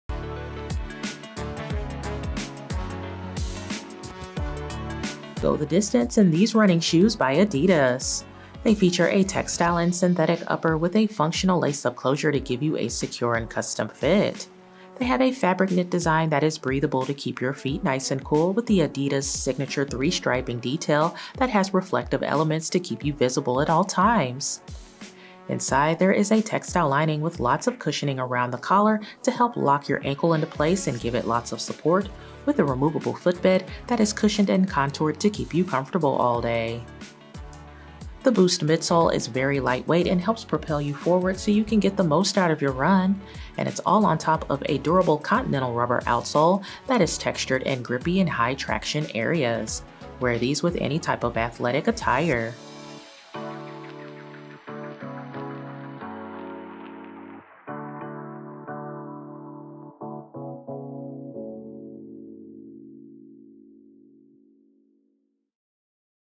adidas Running Solar Glide 5 SKU: 9584398
Go out for jogs and runs wearing this versatile adidas&reg; Running Solar Glide 5 Running Shoes.
Circular knit textile knit upper.
BOOST midsole.
Regular fit.
adidas LEP 2.0 design.
Lace-up closure.
Extended tongue for added protection.
Continental&trade; rubber outsole.
Imported.